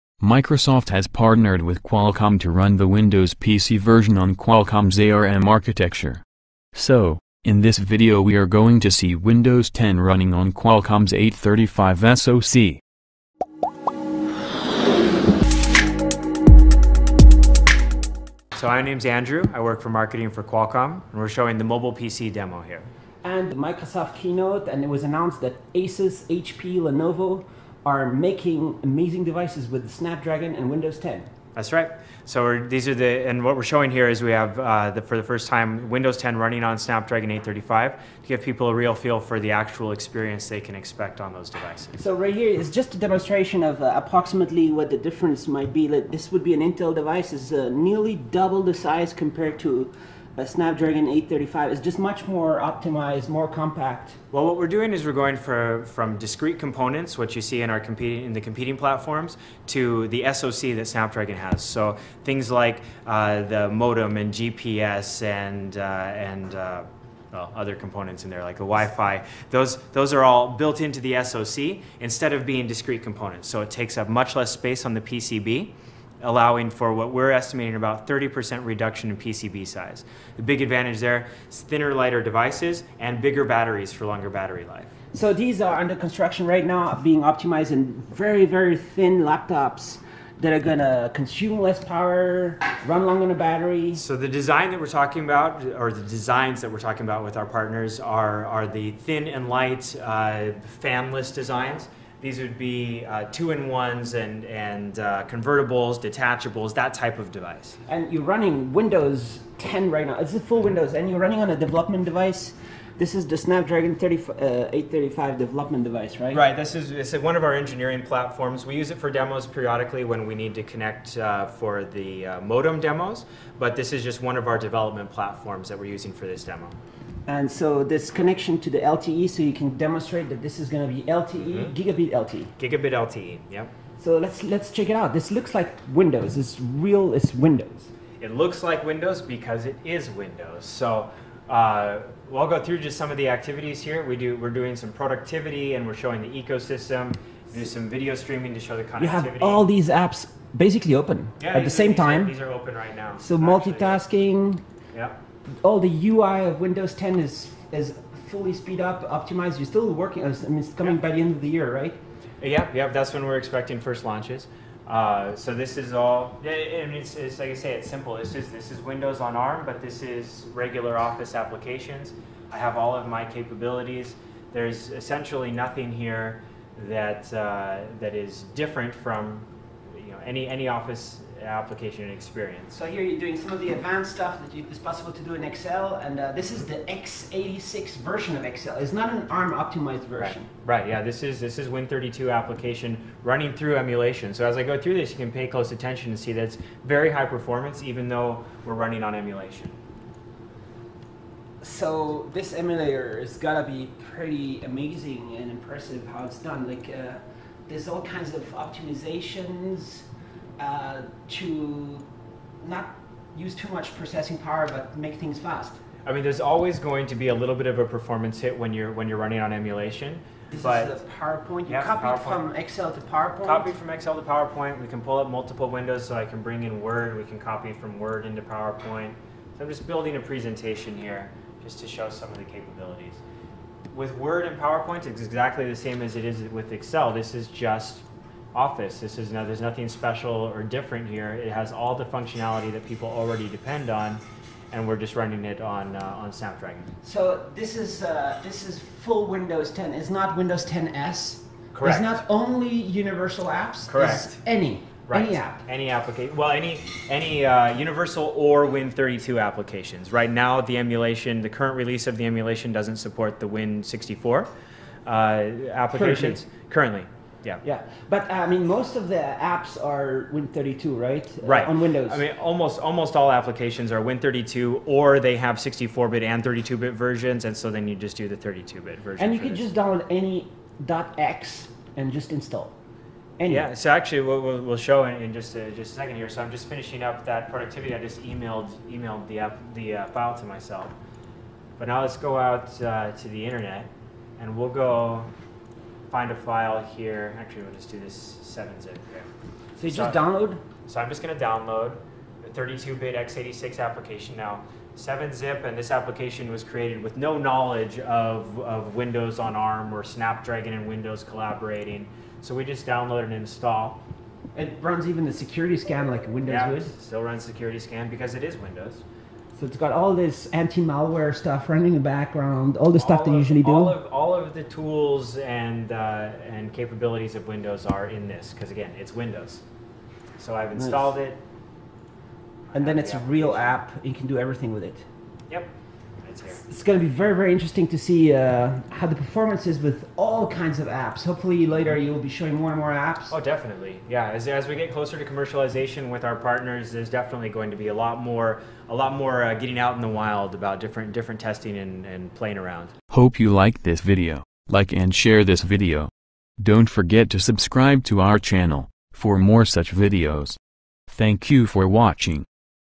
Windows 10 running on Snapdragon 835
Microsoft planning to run its Windows 10 on ARM-based processors. Watch this video to see Windows 10 running on Snapdragon 835 chipset from Qualcomm.

Microsoft Surface Pro 4
HP 15-BE002TX